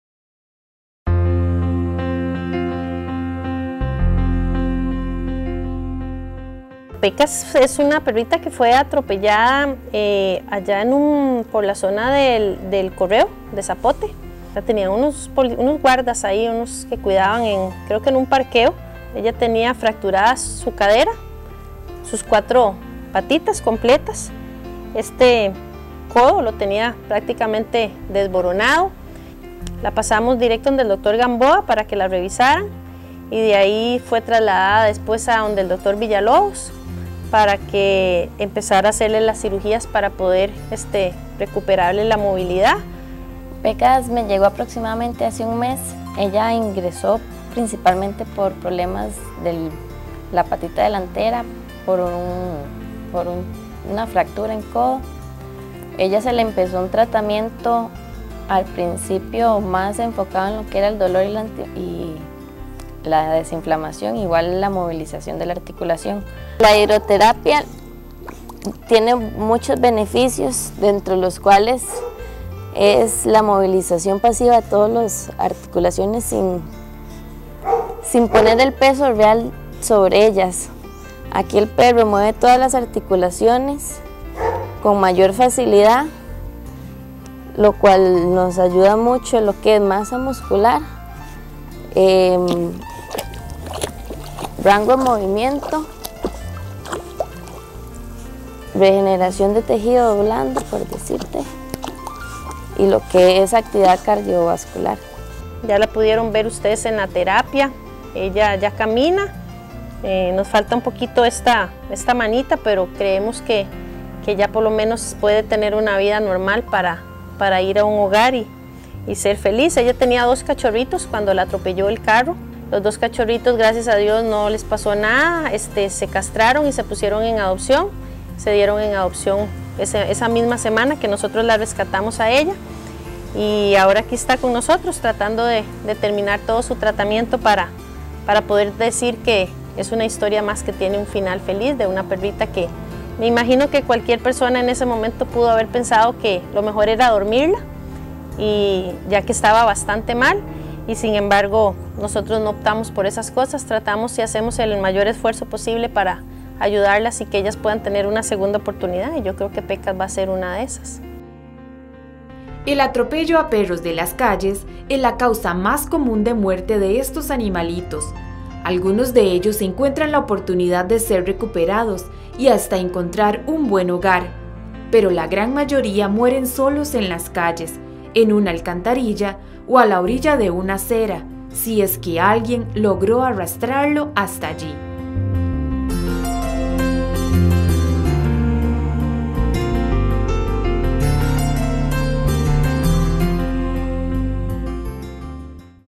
7 Rescate Animal historias por Reino Zoo
Reino Zoo productora costaricense elaboro una seria de ediciones pro bono sobre Rescate Animal, asociacion sin fines de lucro que protege animales abandonados, maltratados en Costa Rica, siendo proactivas con los pocos recursos y donaciones, han logrado darle una segunda oportunidad a
cientos de animales de las calles.

La Telemaraton Canina 2010 fue dedicada a su labor y la primera clinica veterinaria sera para Rescate Animal.